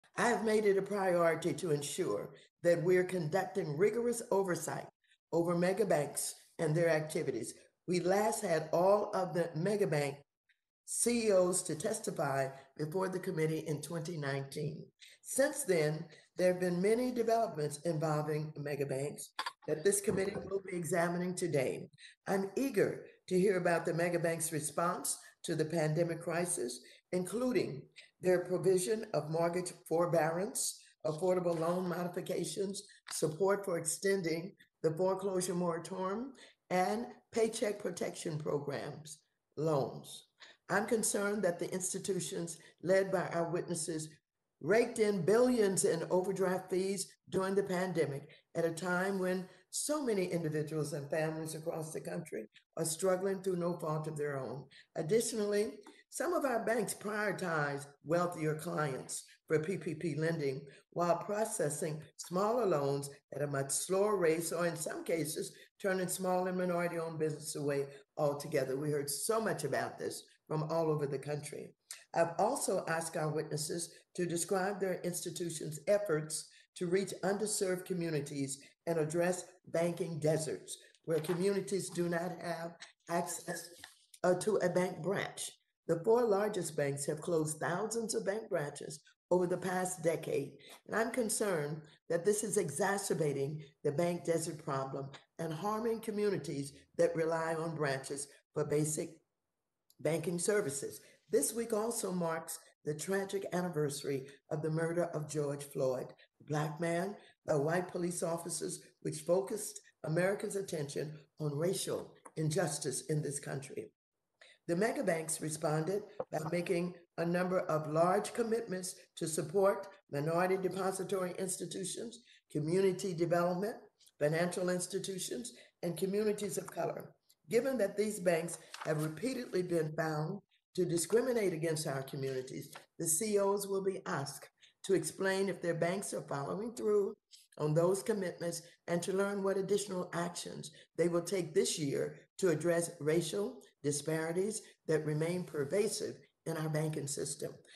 'Megabanks' Raked in Billions in Overdraft Fees During Pandemic: Rep. Maxine Waters
May.27 -- House Financial Services Committee Chairwoman Maxine Waters rebuked Wall Street CEOs for charging overdraft penalties during the pandemic and contributing to “banking deserts” by shutting branches in underserved communities. She opened up a virtual hearing with six bank CEOs.